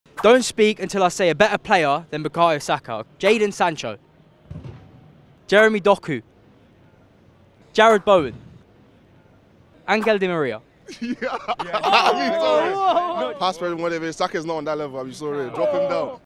STAY SILENT SAKA FOOTBALL CHALLENGE 🤐
Premier League football fans take on the don't speak soccer challenge... stay quiet until you hear a better player than Arsenal and England star Bukayo Saka!